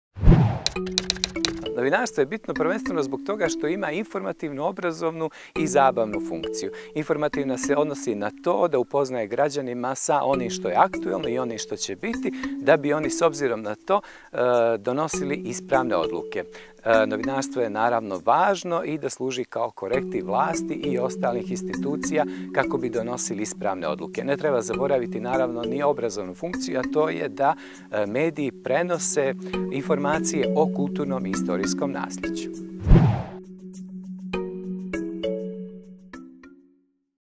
Zašto je novinarstvo važno? - dr sci. Miomir Maroš, komunikolog i urednik emisije u TVCG
Kampanja „Novinarstvo je važno!“ ima za cilj da ukaže na značaj novinarstva i uslove u kojima rade crnogorski novinari/ke. SMCG kampanju sprovodi uz podršku International Federation of Journalists (IFJ).